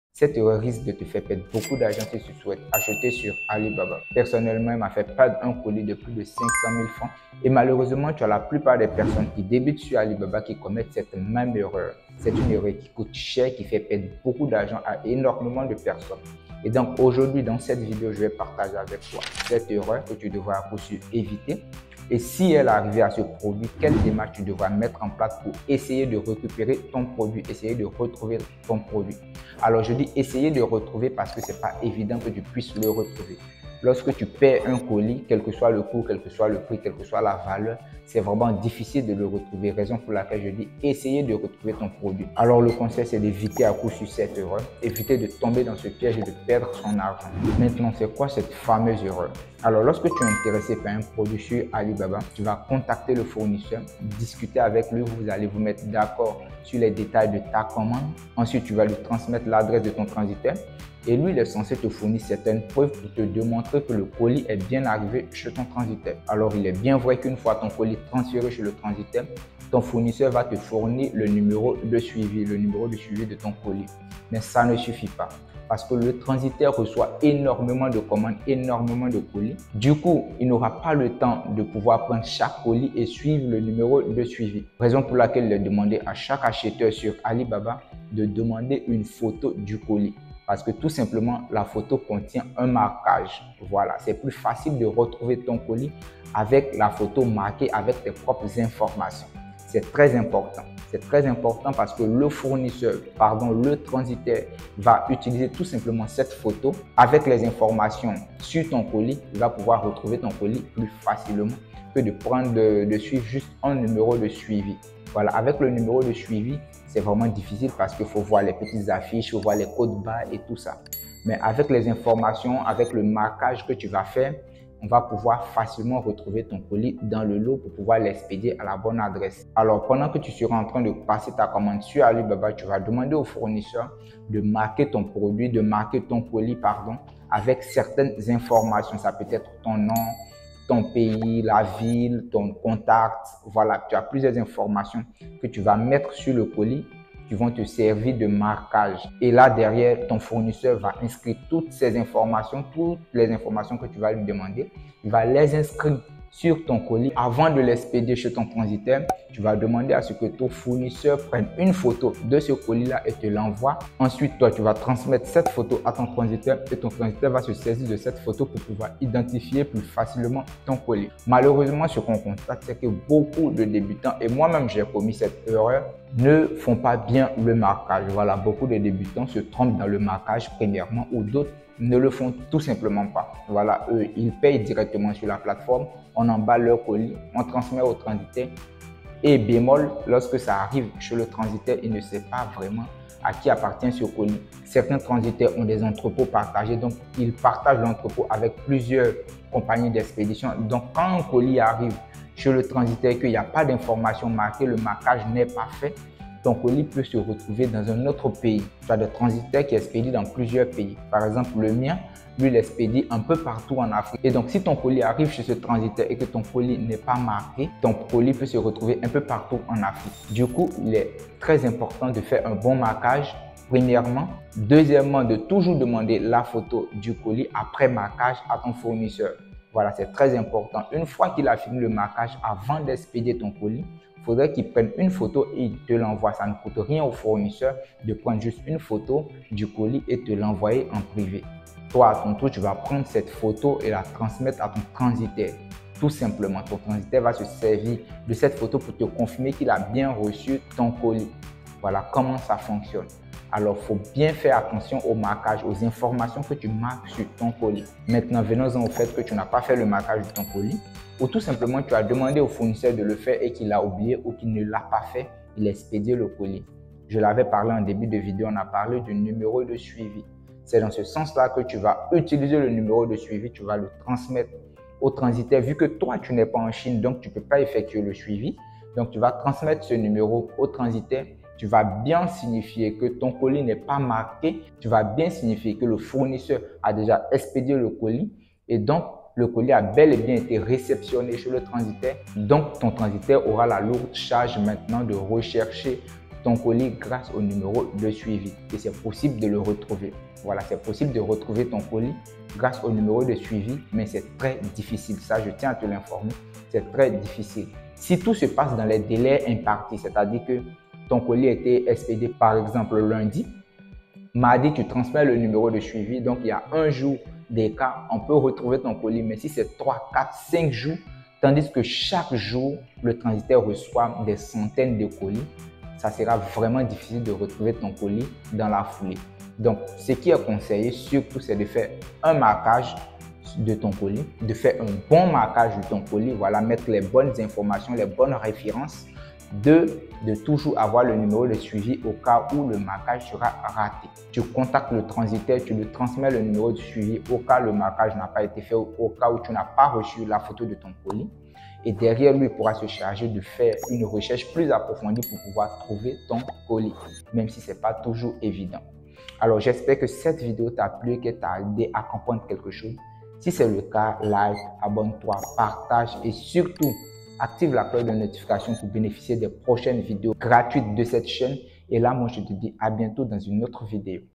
Comment éviter de Perdre ses COLIS sur ALIBABA
Découvre dans cette vidéo ce qu'il faut faire pour Recevoir ses colis en toute sécurité quand tu achète sur Alibaba c'est de loin la Meilleur méthode pour éviter de Perdre ses COLIS sur ALIBABA